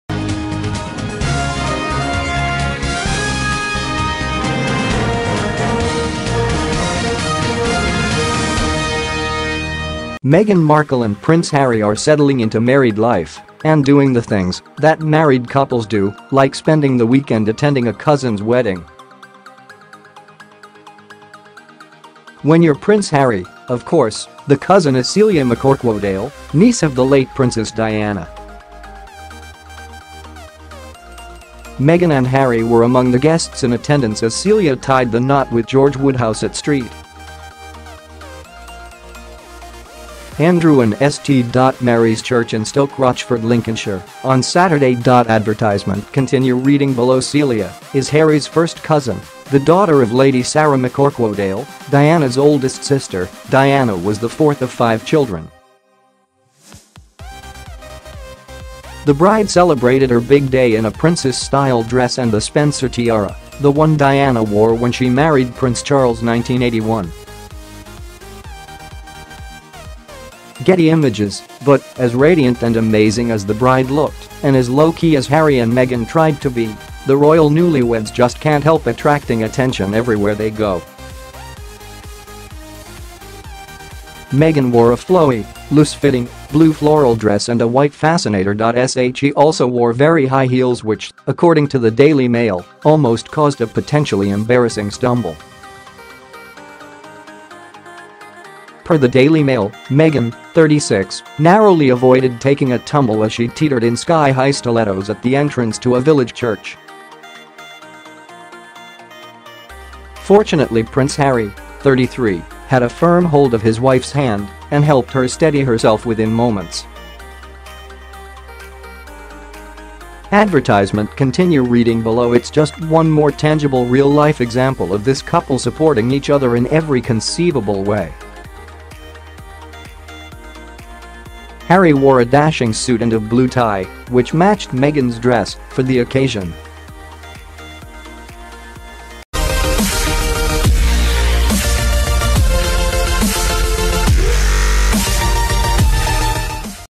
Meghan Markle and Prince Harry Stole the Show at a Royal Family Wedding This Weekend
Meghan Markle and Prince Harry are settling into married life and doing the things that married couples do—like spending the weekend attending a cousin's wedding.When you're Prince Harry, of course, the cousin is Celia McCorquodale, niece of the late Princess Diana.Meghan and Harry were among the guests in attendance as Celia tied the knot with George Woodhouse at St.Andrew and St.Mary's Church in Stoke Rochford, Lincolnshire, on Saturday.Advertisement - Continue Reading Below Celia is Harry's first cousin, the daughter of Lady Sarah McCorquodale, Diana's oldest sister (Diana was the fourth of five children).The bride celebrated her big day in a princess-style dress and the Spencer tiara—the one Diana wore when she married Prince Charles 1981.Getty Images But, as radiant and amazing as the bride looked and as low-key as Harry and Meghan tried to be, the royal newlyweds just can't help attracting attention everywhere they go.Meghan wore a flowy, loose-fitting, blue floral dress and a white fascinator.She also wore very high heels which, according to the Daily Mail, almost caused a potentially embarrassing stumble.Per the Daily Mail: "Meghan, 36, narrowly avoided taking a tumble as she teetered in sky-high stilettos at the entrance to a village church.Fortunately Prince Harry, 33, had a firm hold of his wife's hand and helped her steady herself within moments."Advertisement - Continue Reading Below It's just one more tangible, real-life example of this couple supporting each other in every conceivable way.Harry wore a dashing suit and a blue tie (which matched Meghan's dress) for the occasion.